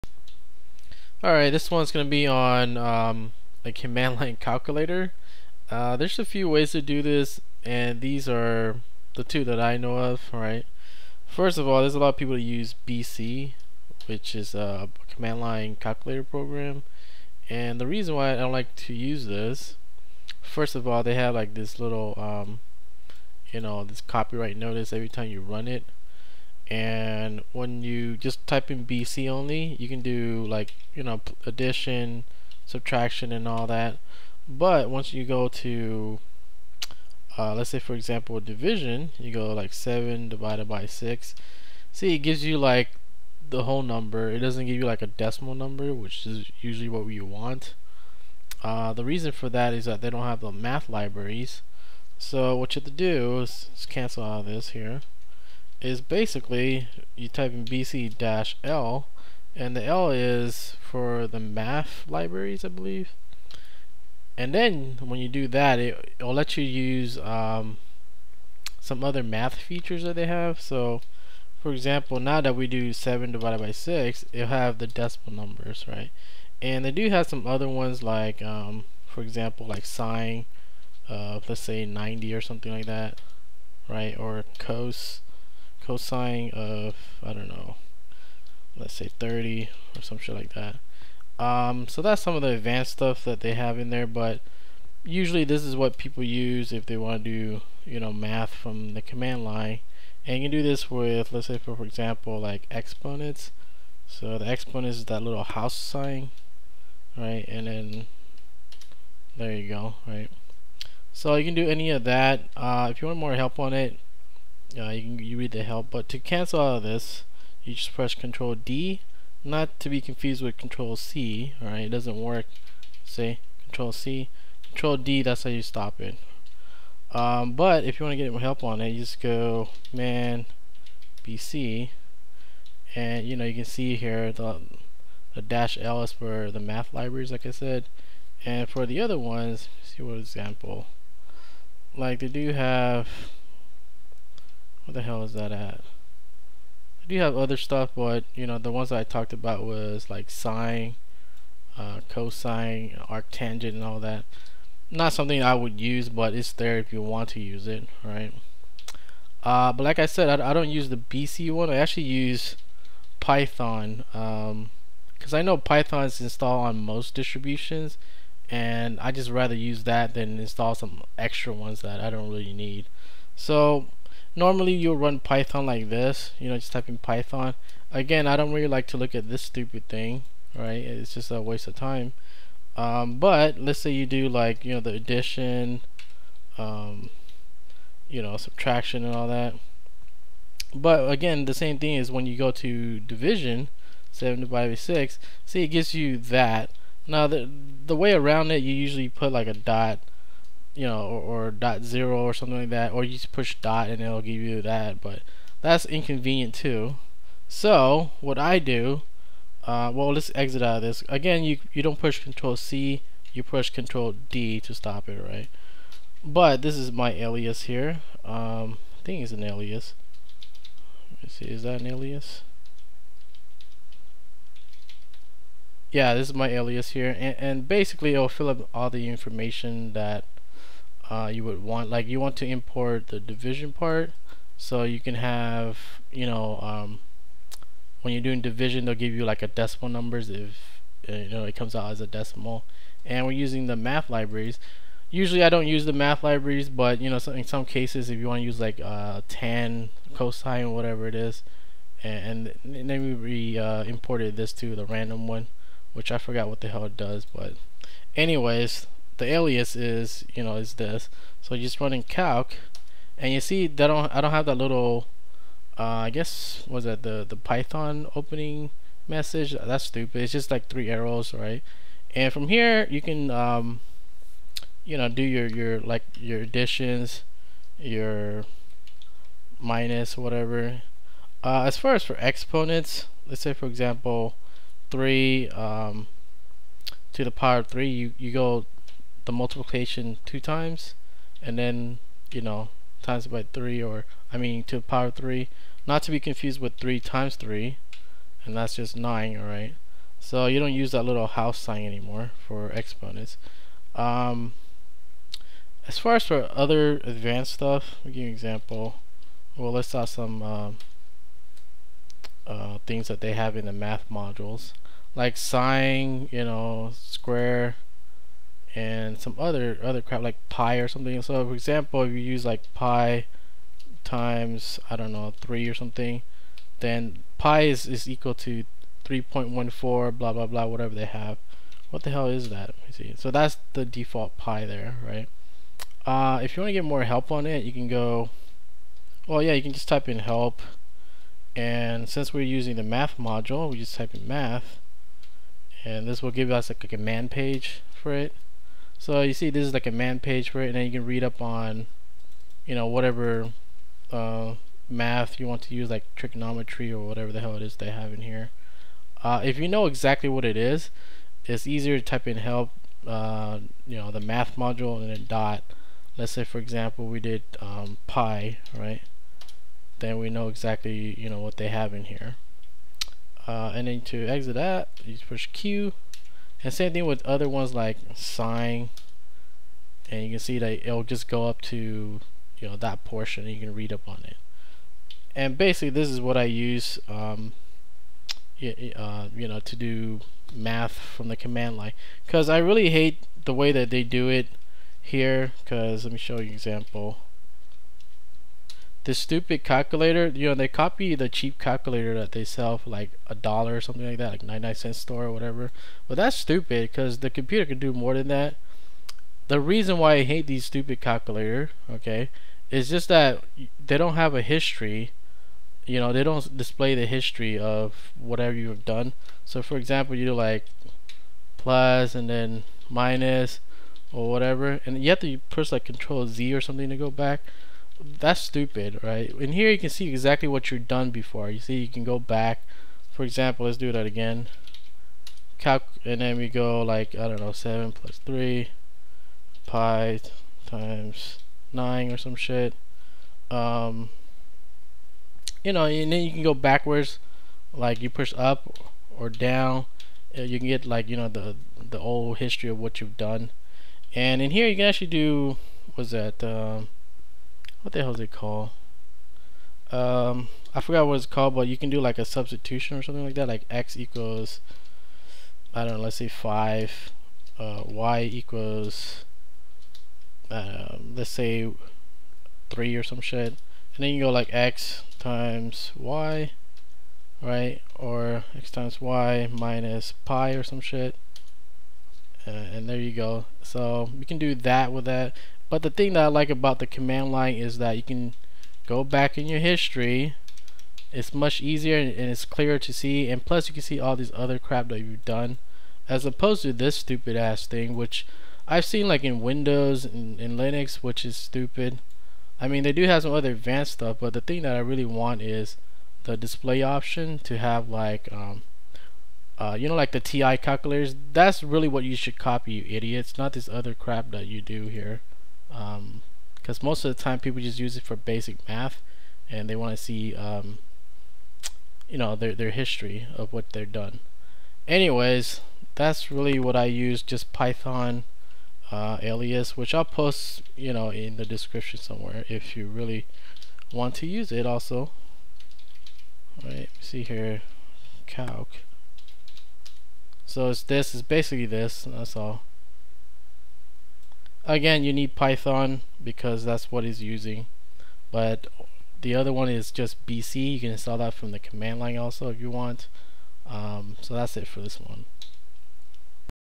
Command Line Calculator - Linux CLI
1. bc - An arbitrary precision calculator language
bc -l

2. python as a calculator
add it to ur bashrc / zshrc

alias calc='python -ic "from __future__ import division; from math import *; from random import *"'

dir()
help('module')

Ctrl+D to exit